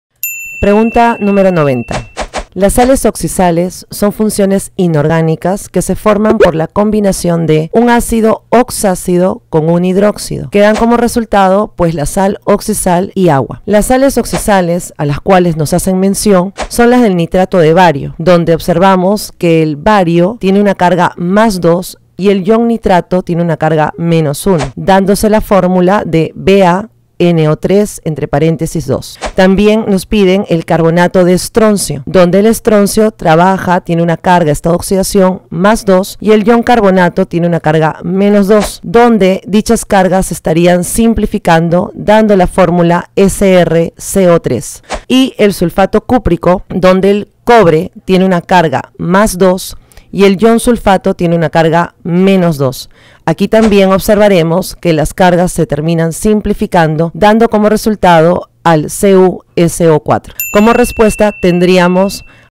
Solucionario San Marcos 2020-1 | Química - Problema 90 (Nomenclatura inorgánica)
¡100 VIDEOS! Resolución de cada problema en menos de 1 minuto.
Solucionario San Marcos 2020 - 1 | Área C
Física - Problema 90
Tema: Nomenclatura inorgánica